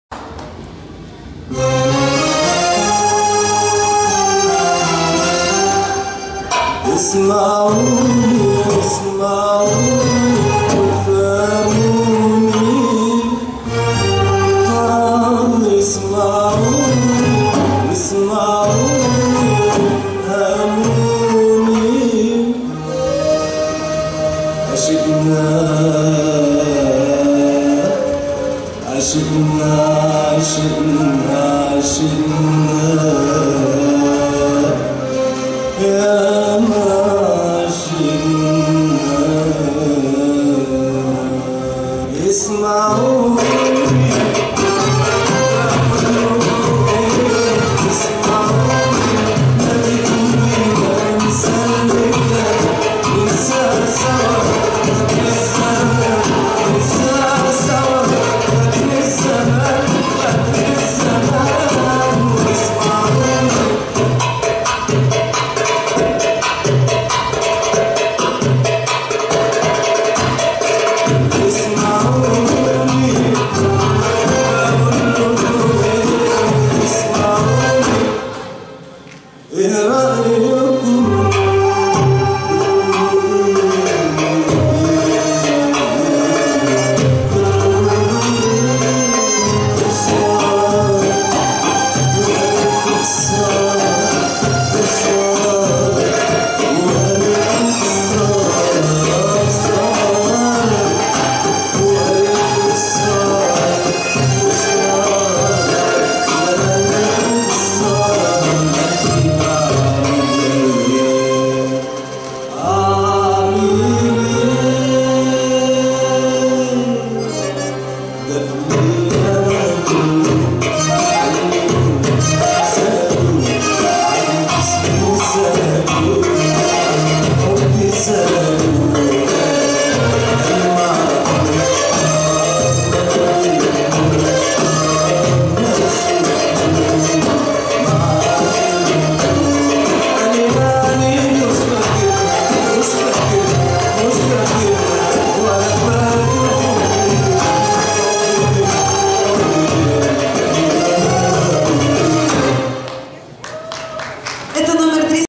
Яна Иванова. Фестиваль-конкурс МАГИЯ 2015
VII Международный конкурс-фестиваль восточного танца “Магия”, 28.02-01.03, Нижний Новгород.
Организаторы: Общероссийская танцевальная организация.
Рейтинг России категории "A".
Номинация - Юниоры открытый класс финал.